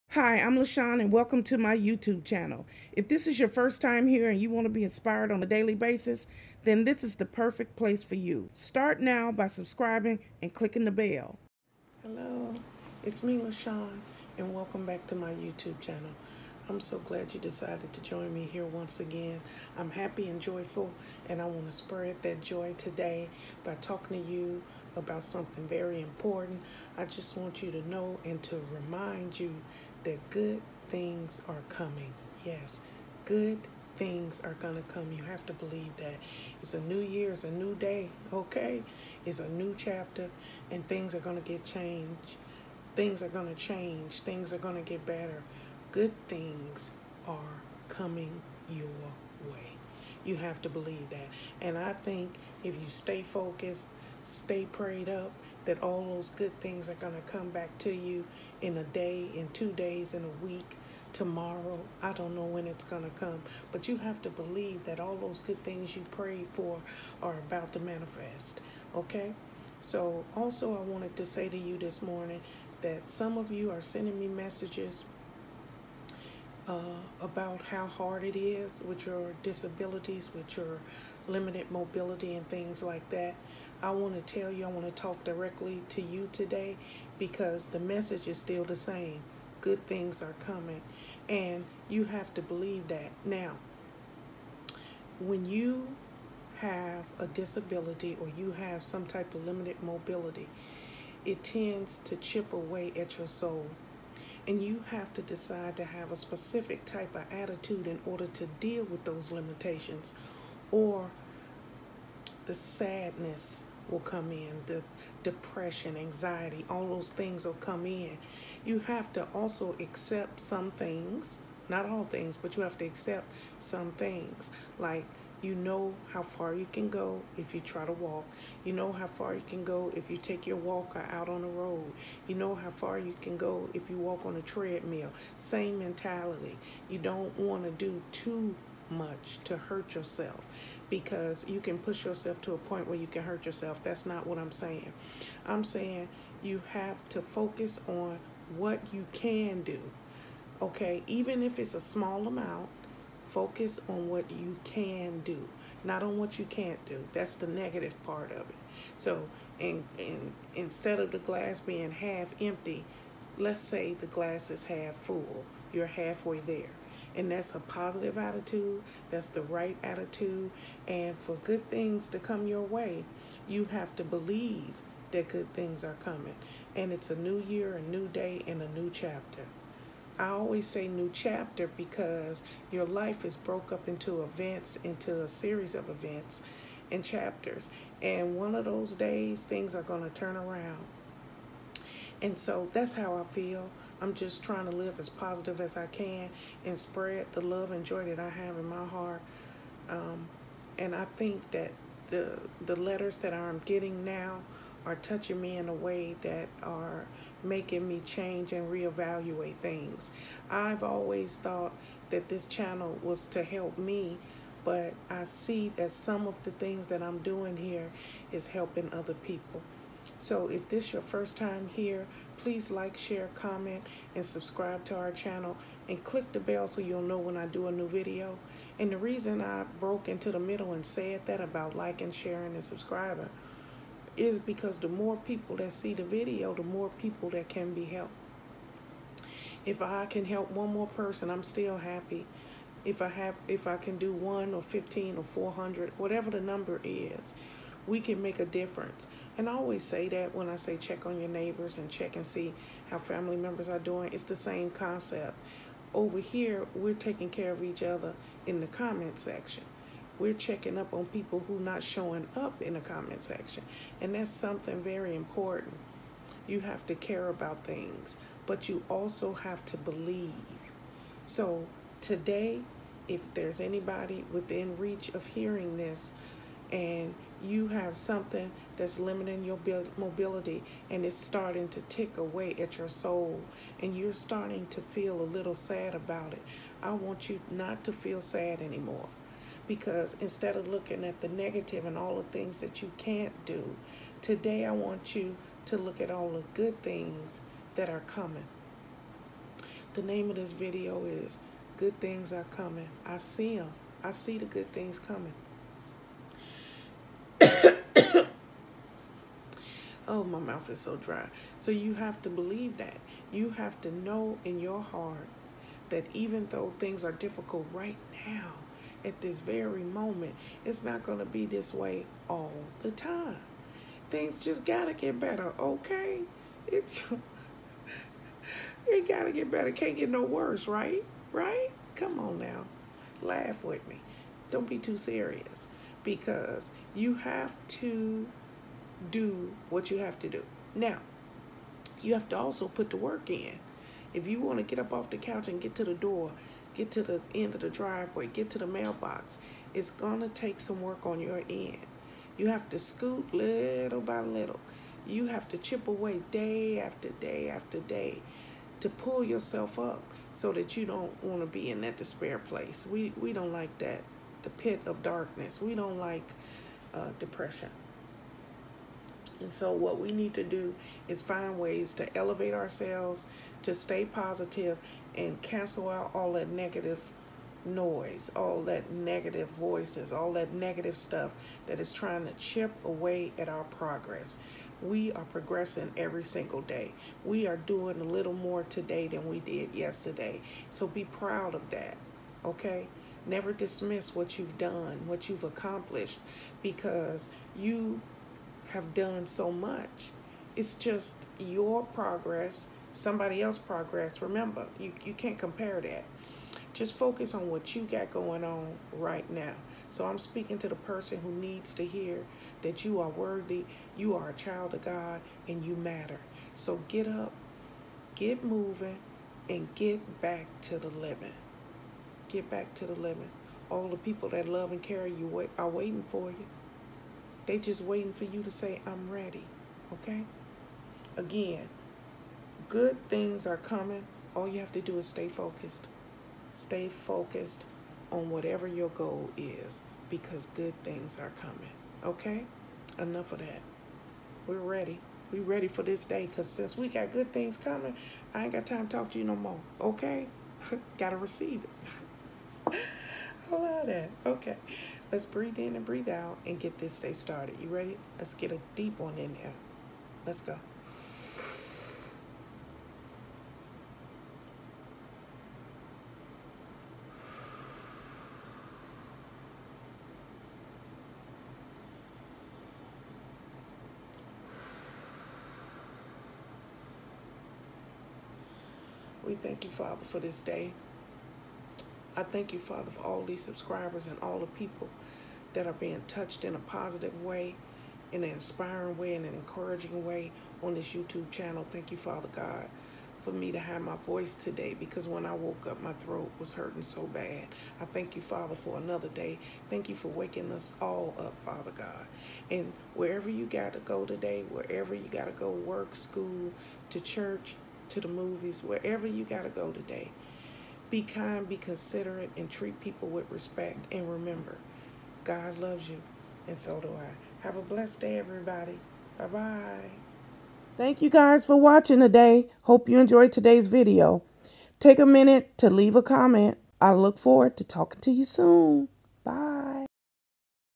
Even though You May Feel Overwhelmed Good Things Are Coming
I have a new video for you today. the name of todays video is called Even though You May Feel Overwhelmed Good Things Are Coming. You have to believe through prayer your life can change for the better. I appreciate all of the support. Thank you and God Bless.